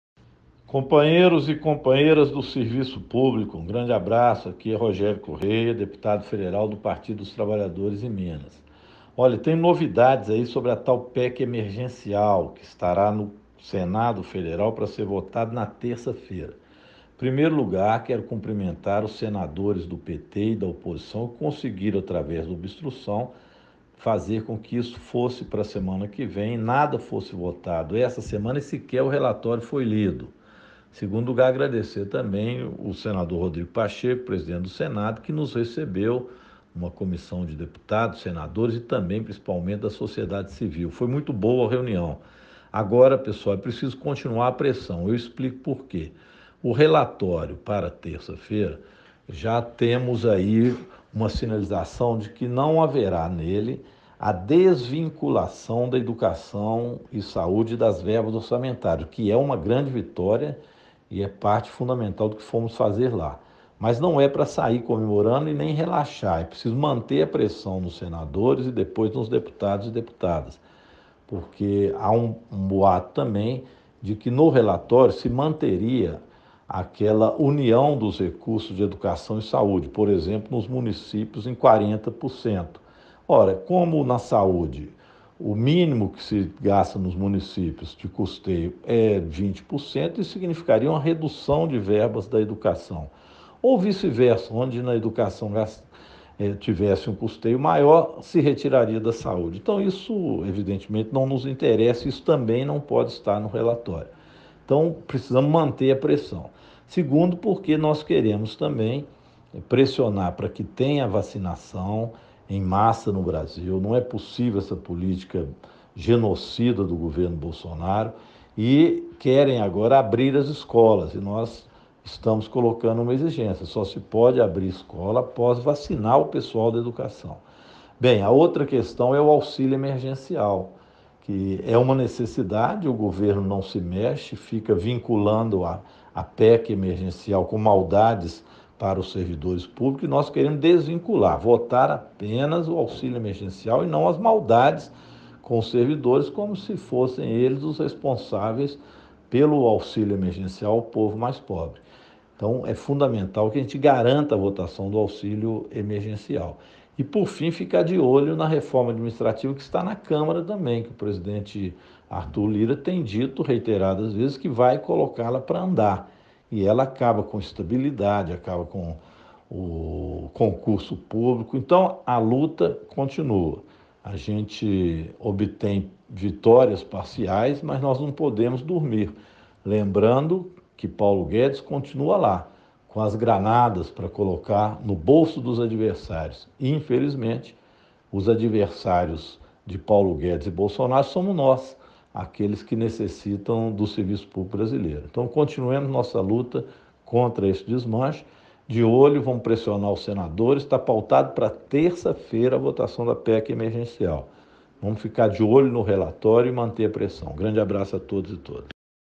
ATENÇÃO: votação da PEC emergencial será na terça-feira
ATENÇÃO! VOTAÇÃO DA PEC EMERGENCIAL NA TERÇA-FEIRA. ENTENDA
A votação da tal PEC Emergencial com que o governo Bolsonaro e o Centrão tentavam, e ainda tentam, retirar dinheiro dos servidores e da educação/saúde, está marcada para esta terça-feira. Foi um importante passo adiar para a próxima semana. Mas não tem nada ganho, é preciso manter a mobilização. Confira agora o áudio que o deputado federal Rogério Correia (PT-MG) gravou para entender melhor. Se puder, por favor compartilhe. Obrigado!